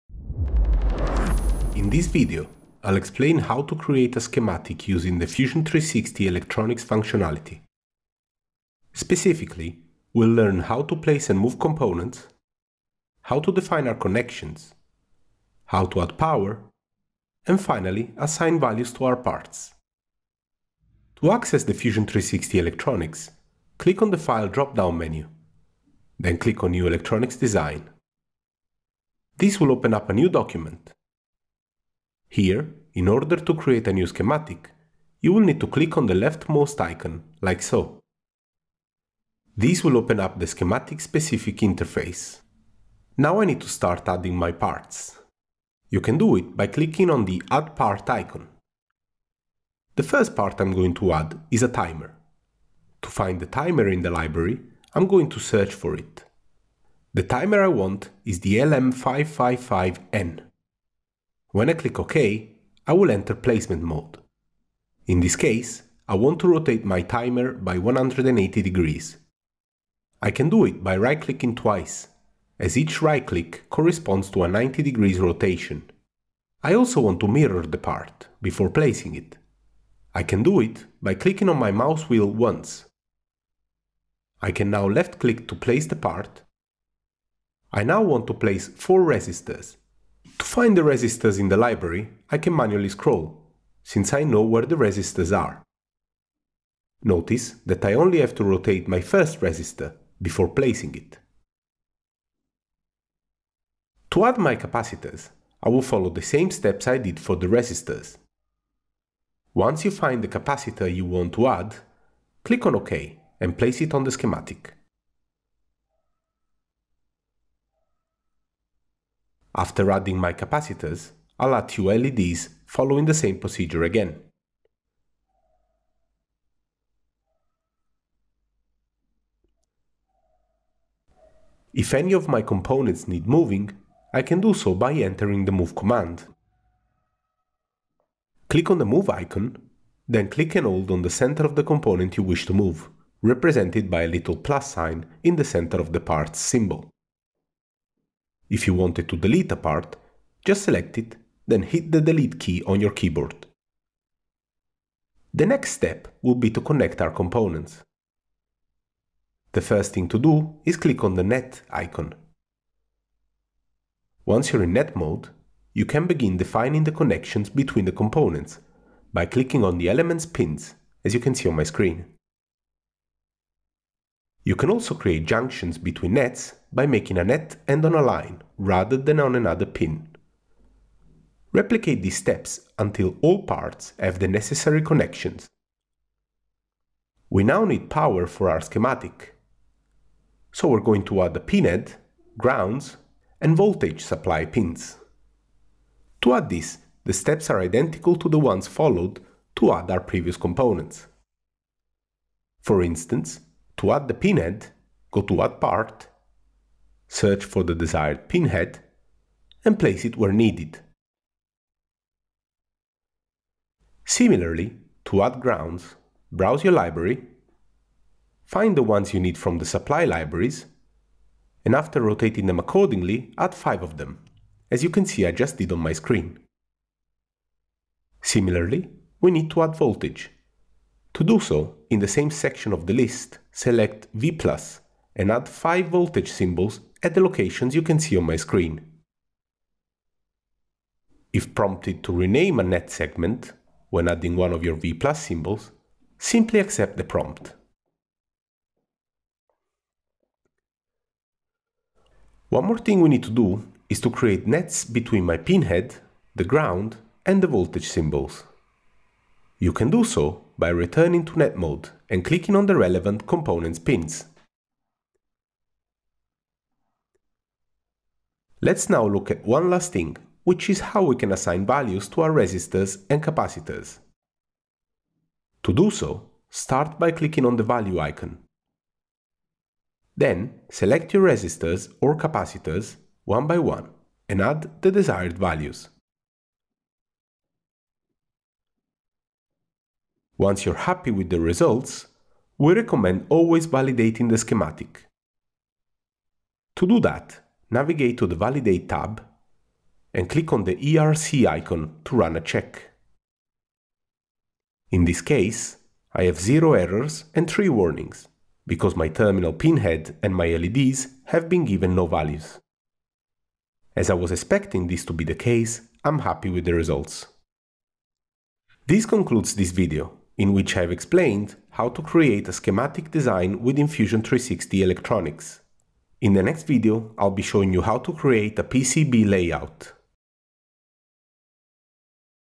Electronics Schematic Tutorial Walkthrough
***if you get stuck trying to find that first component, add a "linear" library from the library manager by going to: Library tab - Library Manager - Available - find "linear" - then click "Use"***

In this video, we'll walk you through the steps related to the documentation tutorials, found here:

Make sure to see part 2 for the PCB layout!